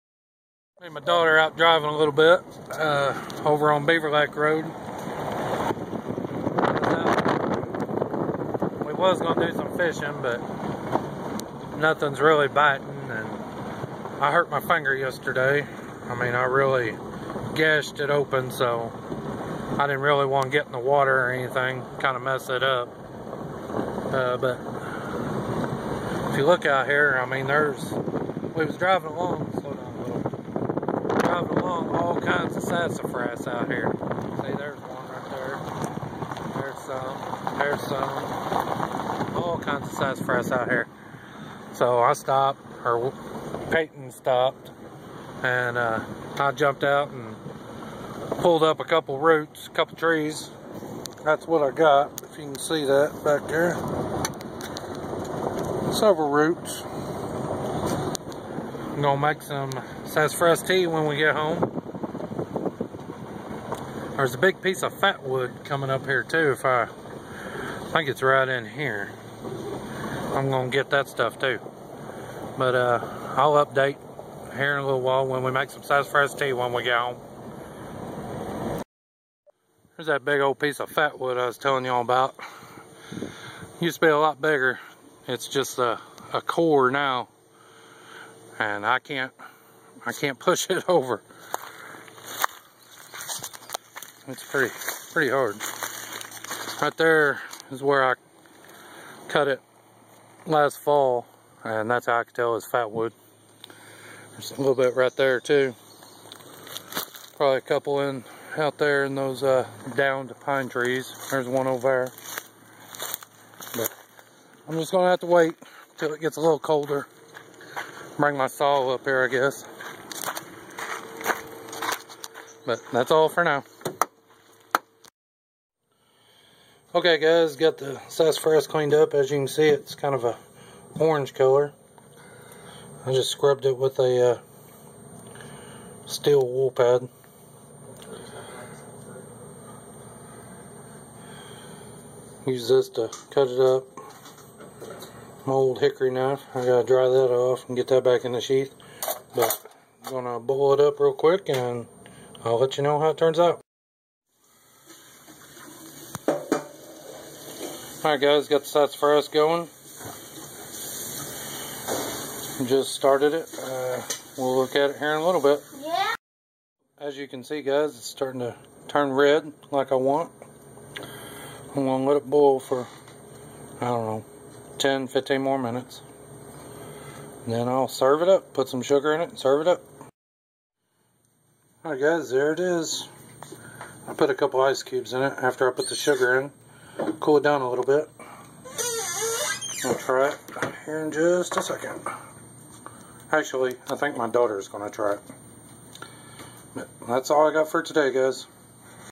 Sassafras and fatwood
Lance and Payt enjoying some sassafras tea.